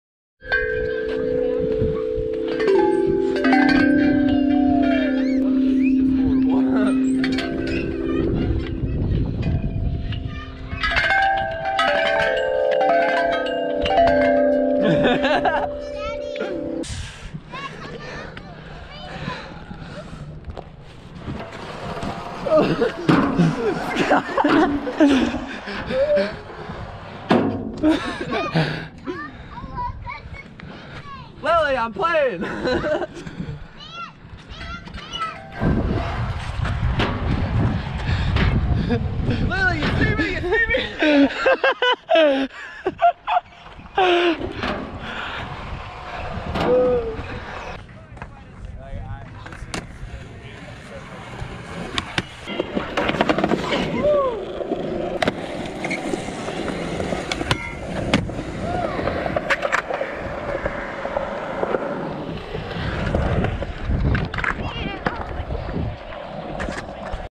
Jason McMahan Waynesville NC SKATE PARK / Rec center
Horsing around in Waynesville NC at the Rec Park